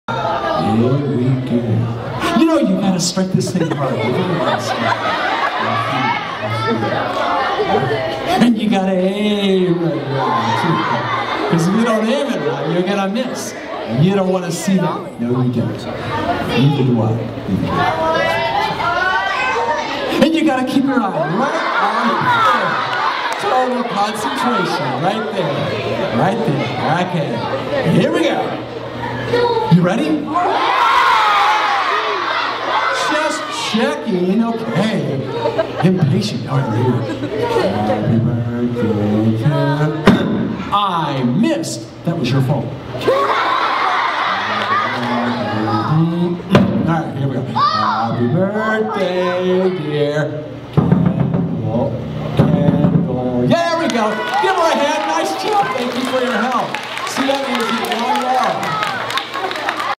Blowing out a Candle with a Sound Cannon
Yes, you can blow out a candle using sound waves. This fun little routine is from my Math, Science & Technology presentation. Special thank you to Doris Sterling for playing along!